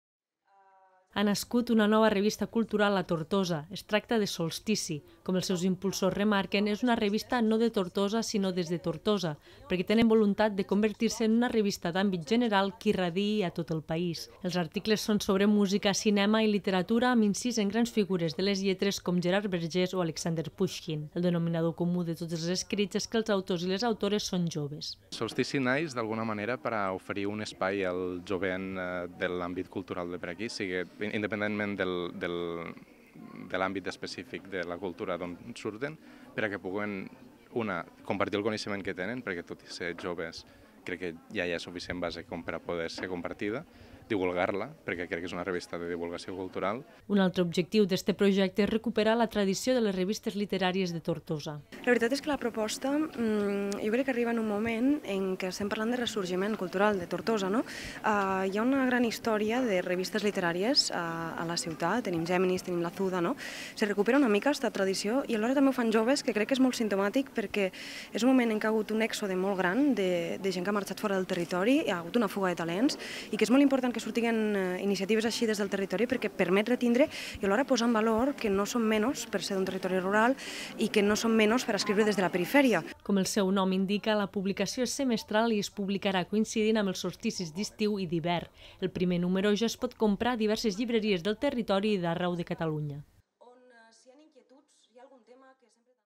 Naix 'Solstici', una nova revista cultural
‘Solstici’ és la nova revista de divulgació cultural feta des de les Terres de l’Ebre, però amb voluntat d’abraçar temes que van més enllà del territori. Així ho han remarcat els seus impulsors en la presentació del primer número de la publicació este dimecres a la Biblioteca Marcel·lí Domingo de Tortosa. És una revista escrita per joves que vol recuperar la tradició de les publicacions literàries tortosines.